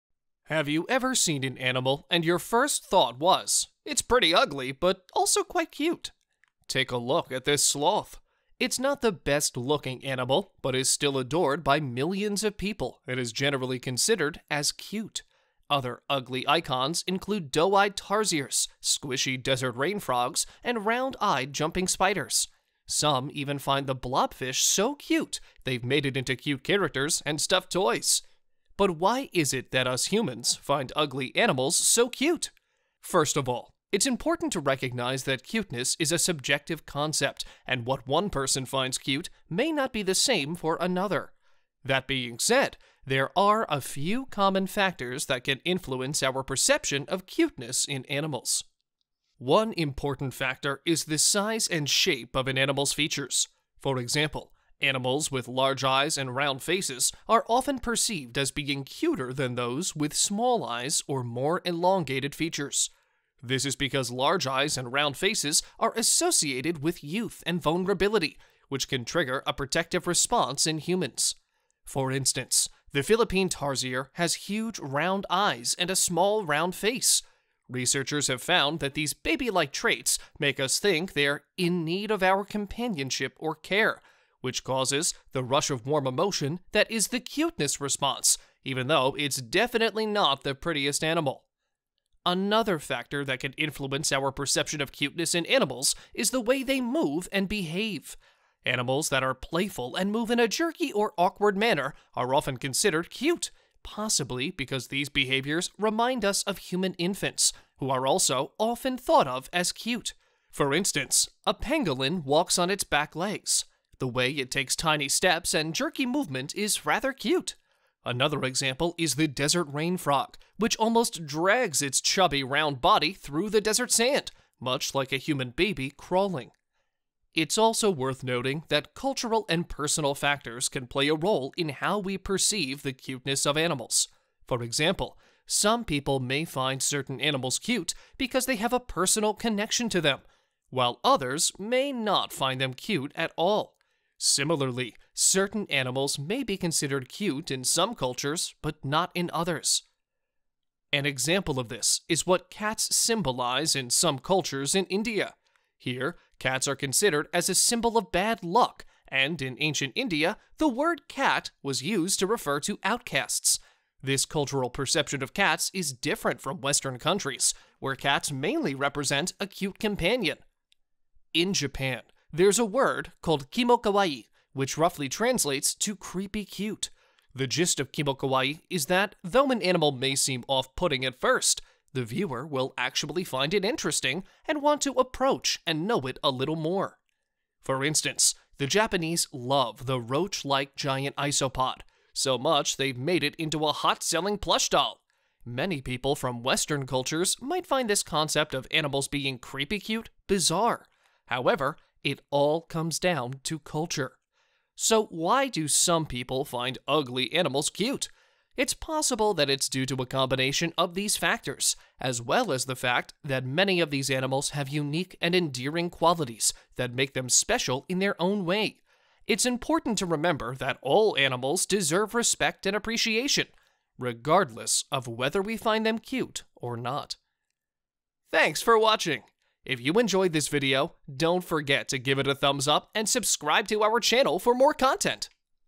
Why Do We Find 'Ugly' Animals Cute?
In this video we'll explore why humans think 'ugly' animals are cute. Have you ever seen an animal and your first thought was: “it’s pretty ugly, but also quite cute”. Take a look at this sloth. It’s not the best-looking animal but is still adored by millions of people and is generally considered as ‘cute’. Other ugly-icons include doe-eyed tarsiers, squishy desert rain frogs and round-eyed jumping spiders. Some even find the Blop fish so cute they’ve made it into cute characters and stuffed toys. But why is it that us humans find ugly animals so cute?